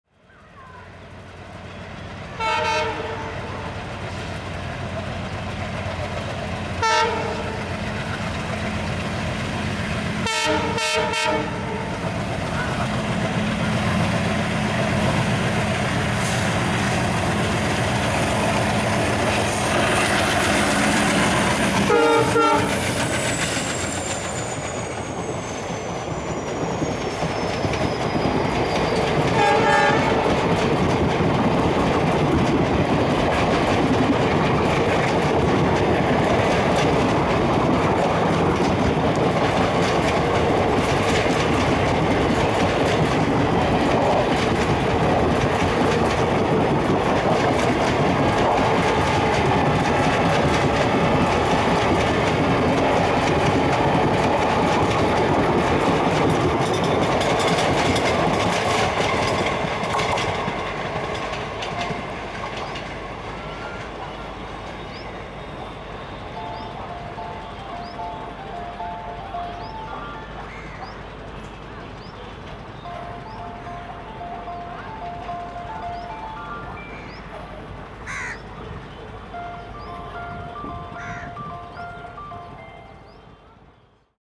[IRFCA] NGC Multiple Unit WDM3D Twin with Guwahati-Barmer/Bikaner Express
New Guwahati MU [Multiple Unit] WDM3D #11561 and WDM3D #11557 powered the 15632 Guwahati-Barmer/Bikaner Express accelerates with the chugging sounds and honks through the level crossing gates at Guwahati City, train was heading toward's Kamakhya Junction.